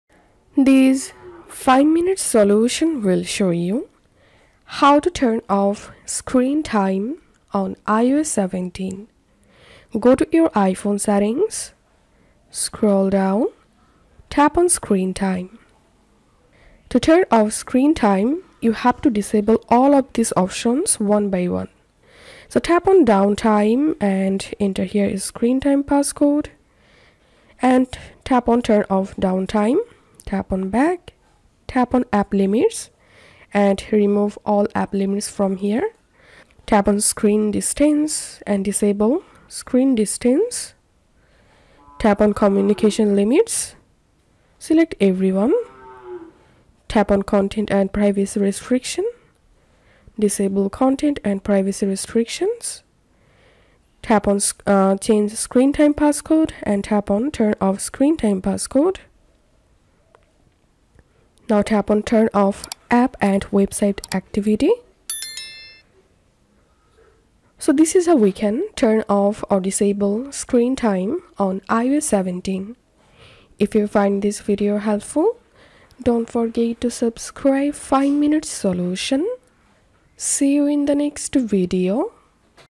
How To Turn Off Screen Time iOS 17 | Disable screen time iPhone
Turning off screen time without passcode on iOS 17 may be a problem that many people are trying to handle. This article will give you several ways to solve the problem.